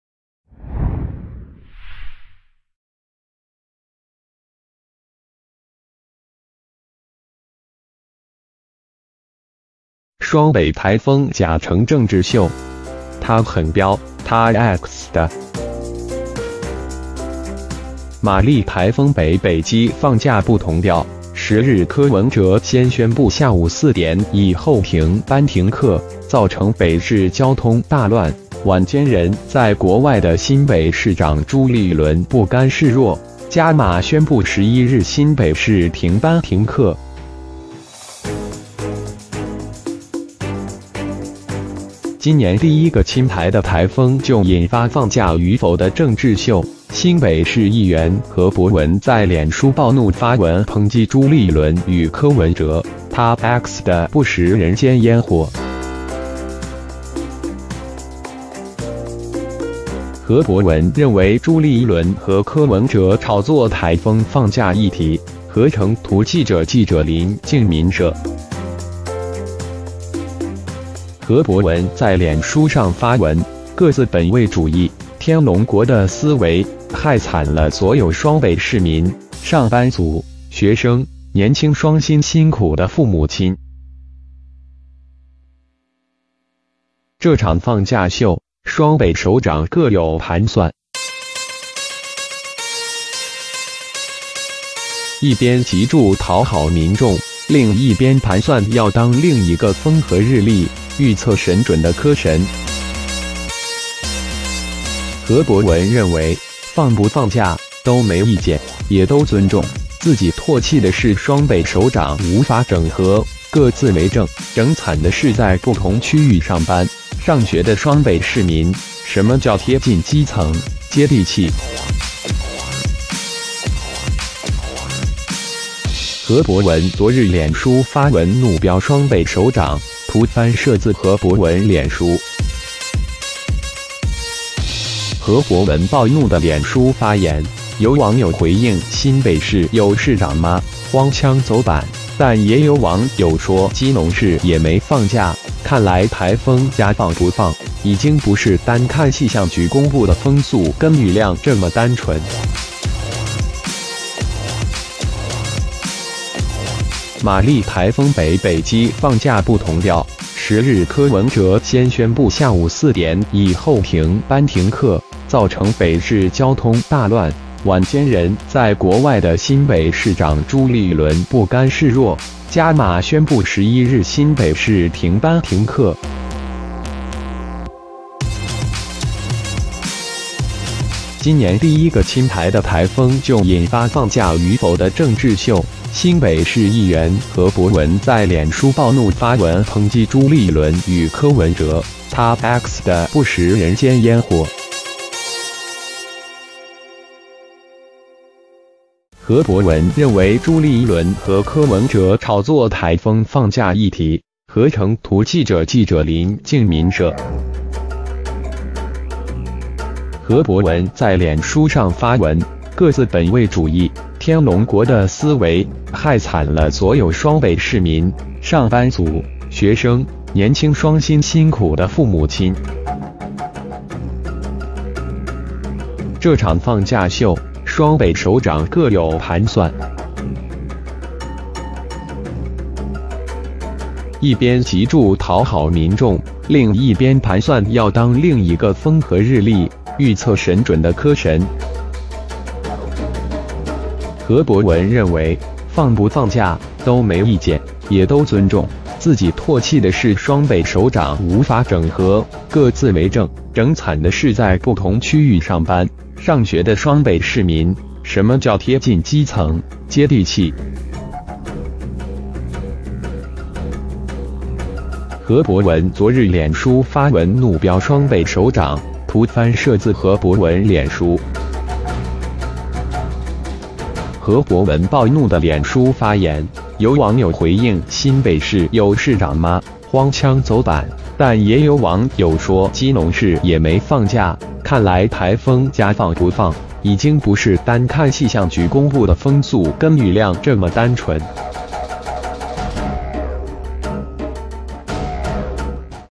雙北颱風假成政治秀！他狠飆：他X的
瑪莉颱風北北基放假不同調，10日柯文哲先宣布下午4點以後停班停課，造成北市交通大亂，晚間人在國外的新北市長朱立倫不甘示弱，加碼宣布11日新北市停班停課，今年第一個侵台的颱風就引發放假與否的政治秀，新北市議員何博文在臉書暴怒發文抨擊朱立倫與柯文哲：「他X的不食人間煙火」。

何博文 ID-1439631

▲何博文認為朱立倫和柯文哲炒作颱風放假議題（合成圖／記者記者林敬旻攝）

何博文在臉書上發文：「各自本位主義，天龍國的思維，害慘了所有雙北市民、上班族、學生、年輕雙薪辛苦的父母親。這場放假秀，雙北首長各有盤算。一邊急著討好民眾；另一邊盤算要當另一個風和日麗、預測神準的柯神。」

何博文認為，放不放假，都沒意見，也都尊重，自己唾棄的是雙北首長無法整合、各自為政，整慘的是在不同區域上班、上學的雙北市民，「什麼叫貼近基層、接地氣」？

何博文臉書 ID-1439630

▲ 何博文昨日臉書發文怒飆雙北首長（圖／翻攝自何博文臉書）

何博文暴怒的臉書發言，有網友回應「新北市有市長嗎」、「荒腔走板」，但也有網友說「基隆市也沒放假」，看來颱風假放不放，已經不是單看氣象